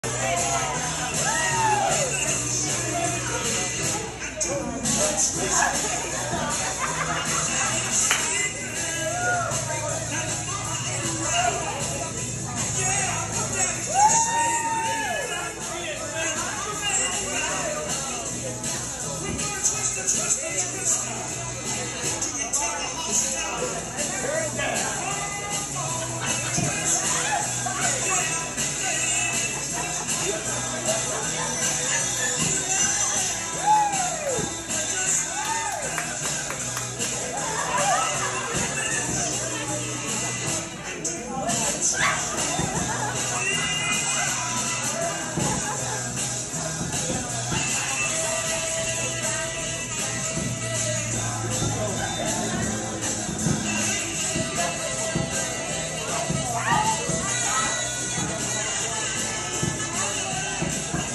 Tommy Twist
Tommy and Deedy do the Twist!